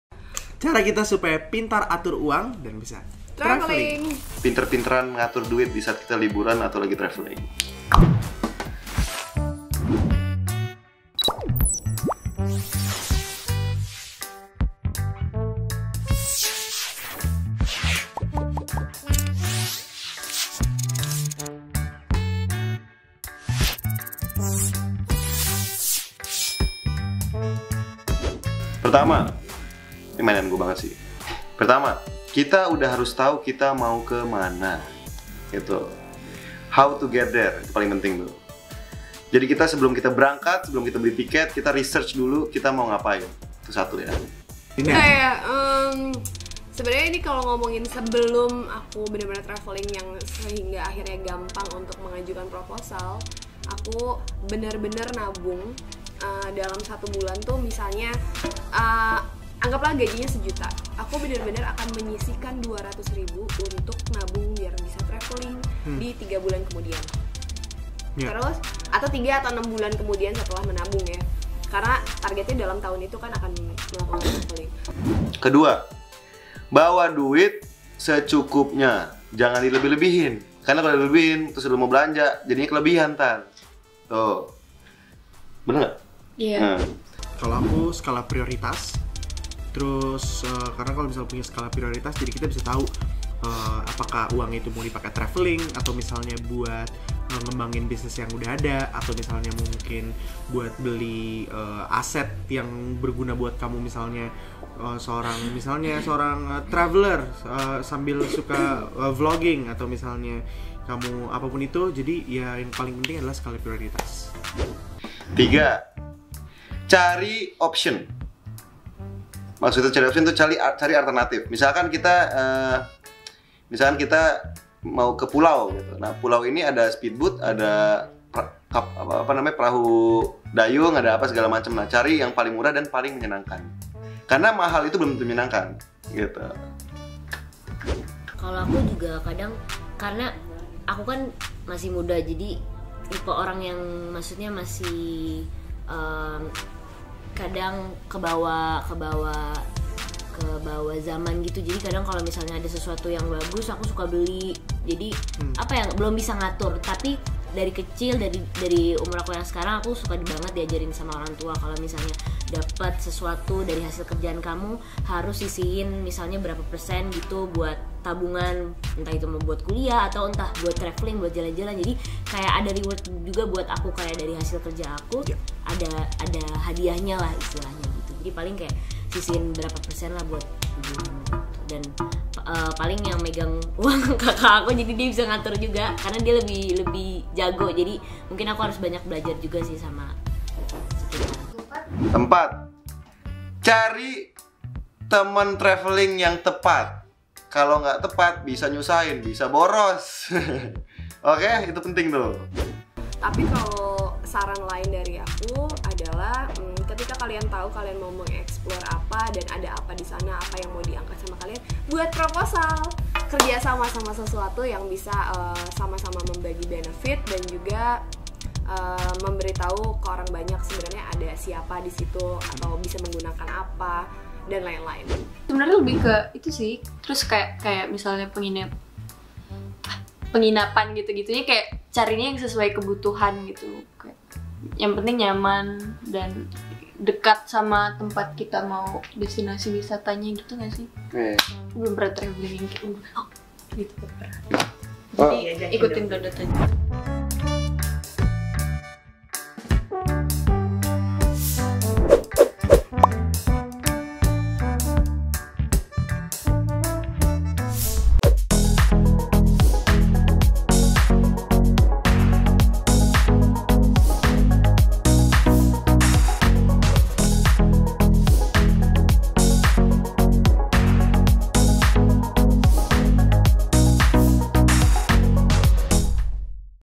Siapkan Uang untuk Liburan Setelah Pandemi ala Artis Muda Indonesia
Sahabat NOVA, berwisata memang membutuhkan pengeluaran yang tidak sedikit, sehingga kita harus mampu PintarAturUang agar kita tidak boros saat pergi berjalan-jalan. Bagaimana caranya supaya traveling kita tetap nyaman tapi juga tidak sampai mengamburkan uang yang berlebihan? Kali ini Adipai DOlken dan Adinda Thomas bagikan tipsnya nih.

0:47 Tips traveling hemat dari Adinda Thomas
1:16 Adipati Dolken liburan dengan cara ini
1:55 Tips traveling dari Adipati dolken

EDITORIAL OFFICE

ADVERTISING